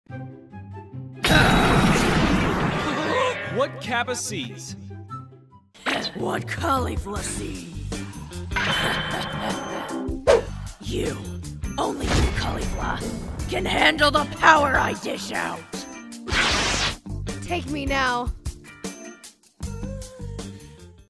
WHAT CABBA SEES, WHAT CAULIFLA SEES! | Dragon Ball Super Comic Dub
Cabba and Caulilfa see Berseker Kale very VERY differently.

Twitter Quote: I just had to quickly get this silly idea out of my head. ~Keereru~ 2022

And we thank you for it! ^-^

Have you ever seen Kale Becomes The Legendary Saiyan! | Dragon Ball Super Comic Dub before watching this video?